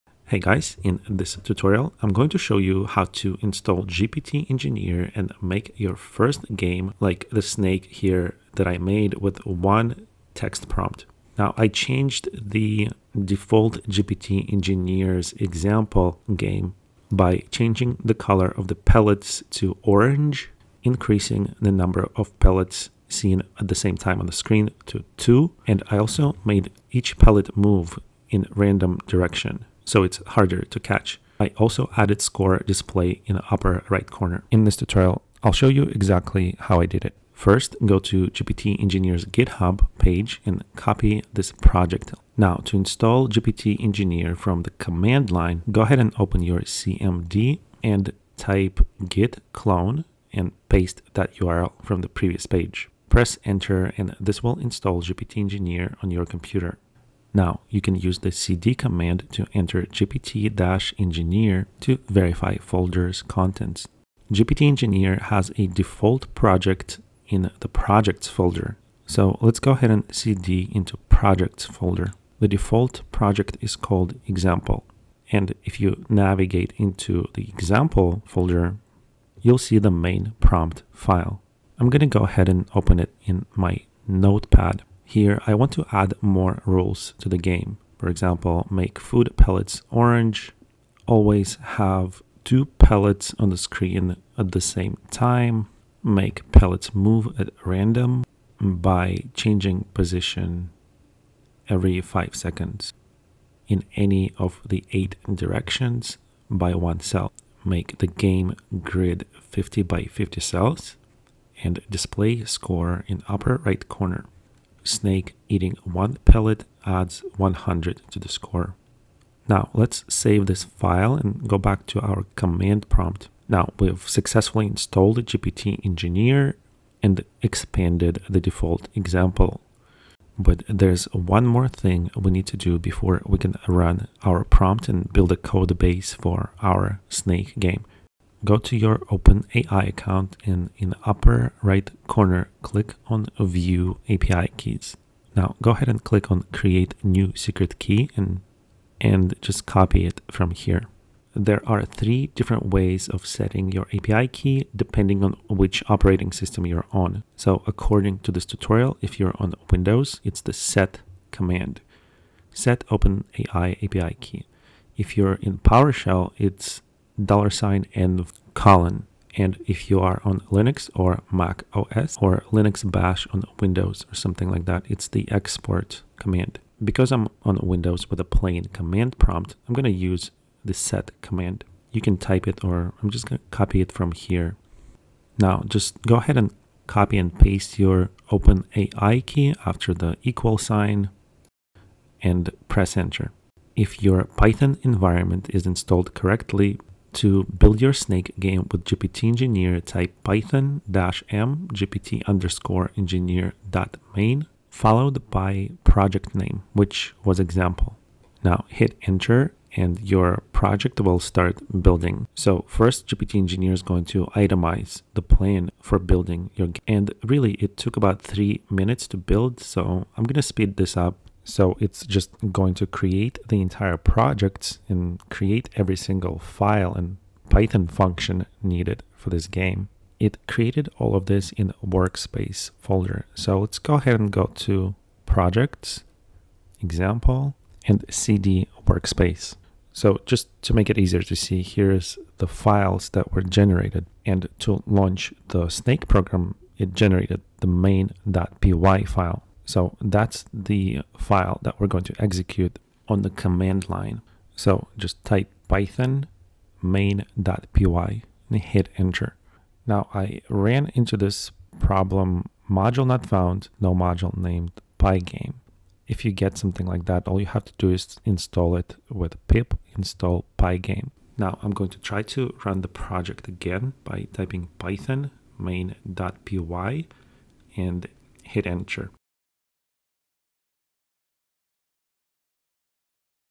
How To Install GPT Engineer (Quick gpt-engineer setup from command line )
(Ask GPT anything for free, with instant response)
(Meet others in our GPT channels and be part of growing community!)

gptengineer gpt4 chatgpt4 How to install GPT engineer? This quick cmd.exe tutorial will walk you through the process of installing and setting up GPT engineer on the command line. Then, we'll build the default snake game with keyboard controls, but also add our own game rules, and add more detail to codebase building instructions file (main_prompt).

00:00 How to install gpt-engineer
00:37 Clone gpt-engineer prior to setup
01:17 Modifying main_prompt file, to add more detailed instructions for building snake game
02:09 Set OpenAI GPT4 API Key on command line
03:13 Building the game project with gpt-engineer
04:12 How to run gpt-engineer project from command line